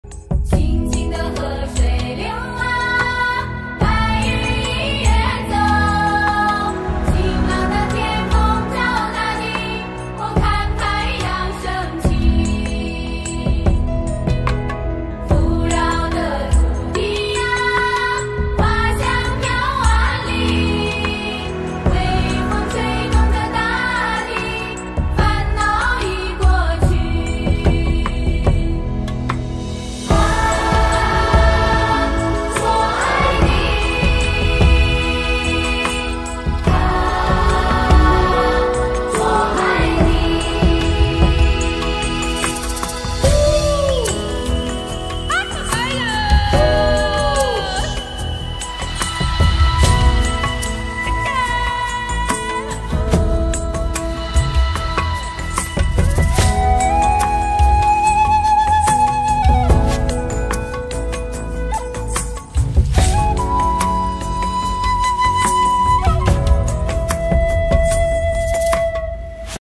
Aerial views of Xiaozhan rice harvest in N China
Xiaozhan rice, which is a popular rice variety in China, is originated in Xiaozhan Town of Tianjin. It has entered harvest season recently. Let’s take a look from a bird's-eye view.